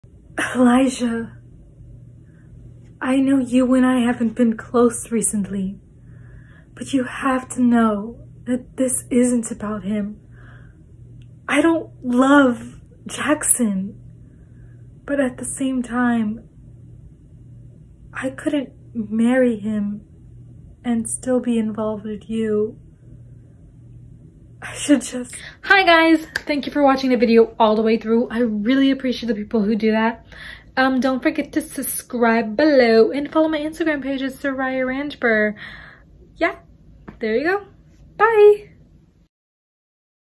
Hayley Talks About Marriage to Elijah Monologue Part 2
I film monologues or short scenes from Movies and TVs or even plays.